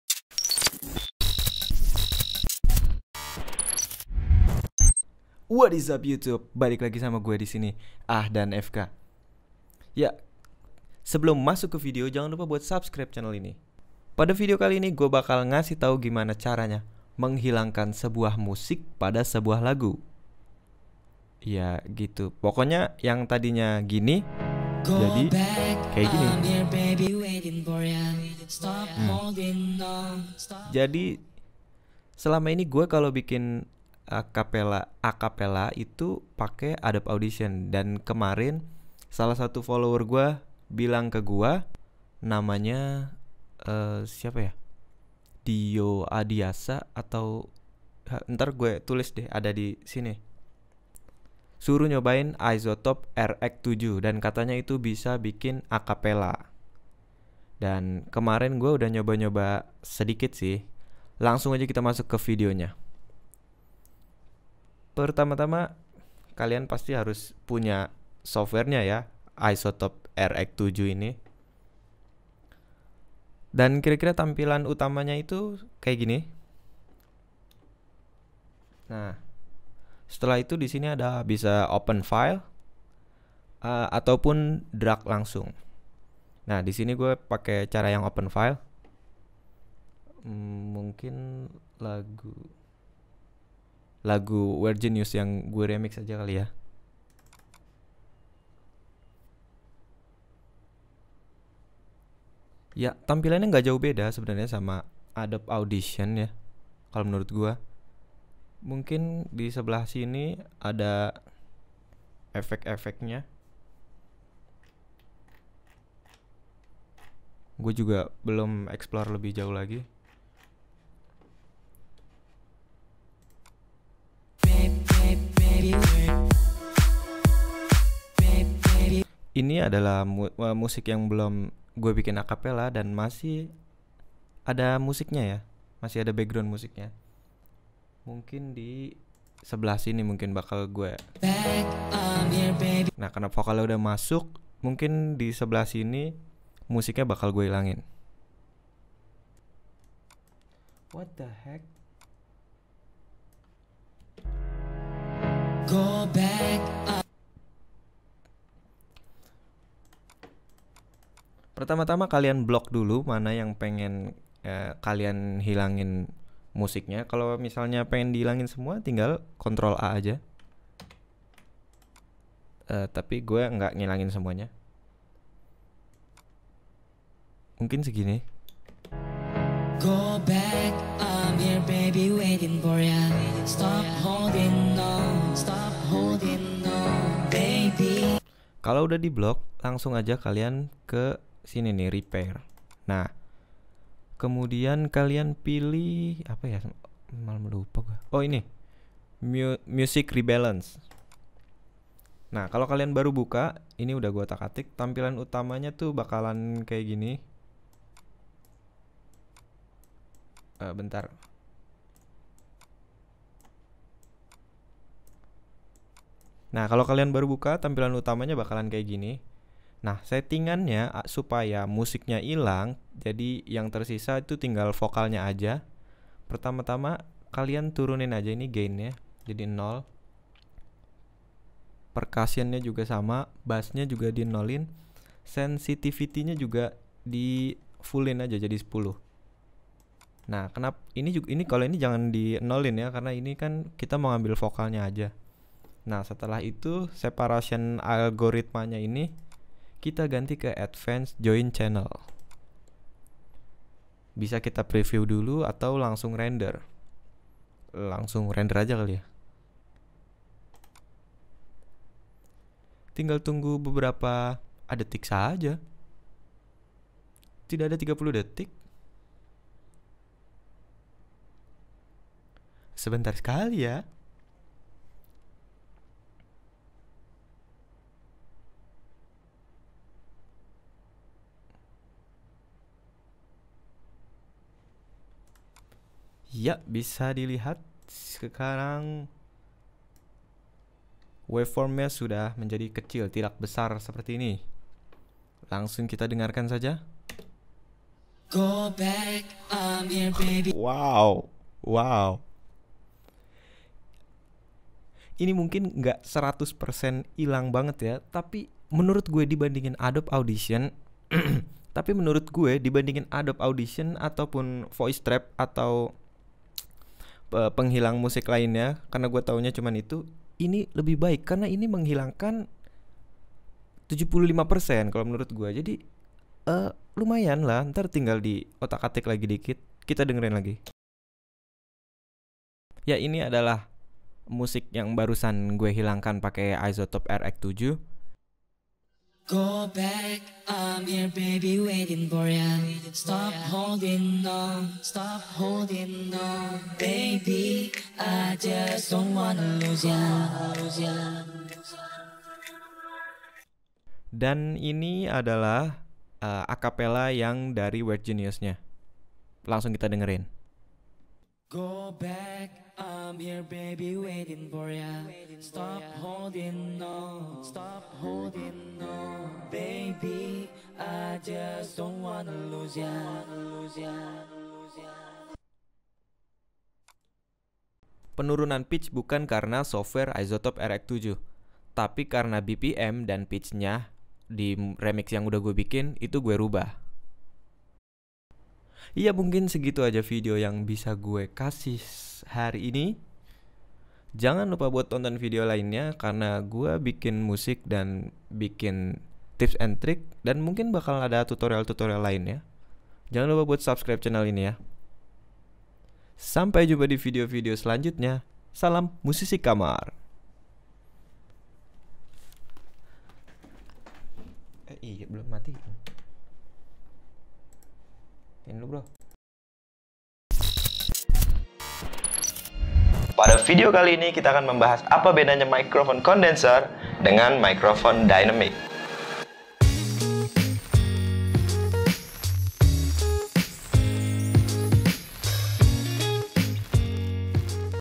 CARA MENGHILANGKAN MUSIK PADA LAGU (ACAPELLA) - IZOTOPE RX7
Kalo lagu yang udah jadi, terus di ambil vokalnya aja bisa gak sih ? Gimana cara bikin Acapella ? Emang bisa ya ? Pake software ini bisa kok.
Tonton video ini sampai habis biar tau

Based In Tangerang Selatan, Indonesia